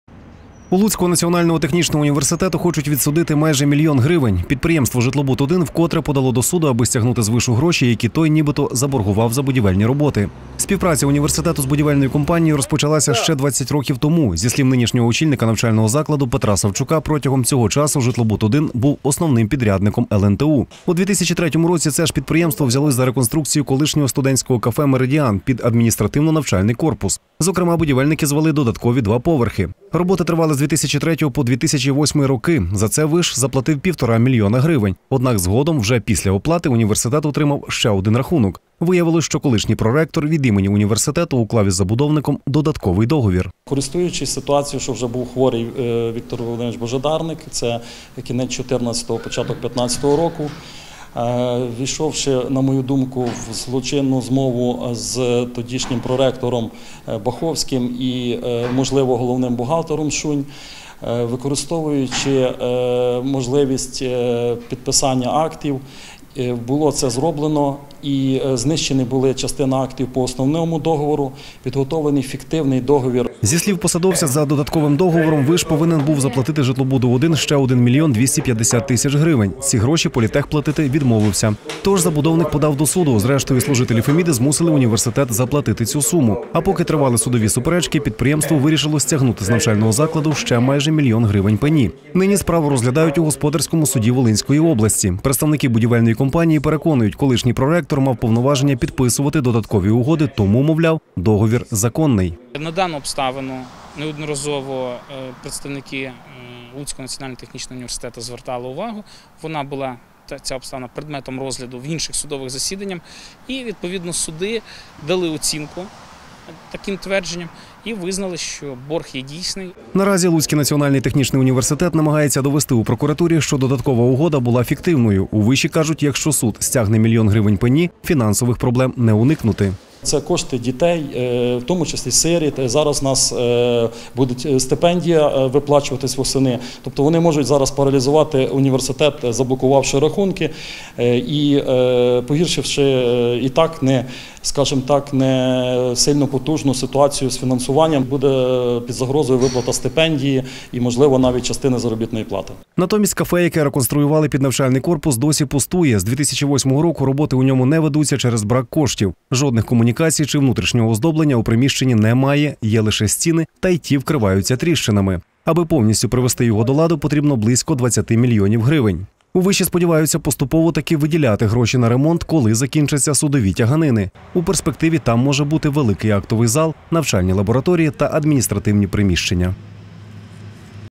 Університетська справа на мільйон
У Луцького національного технічного університету хочуть відсудити майже мільйон гривень. Підприємство "Житлобуд-1" вкотре подало до суду, аби стягнути з вишу гроші, які той нібито заборгував за будівельні роботи.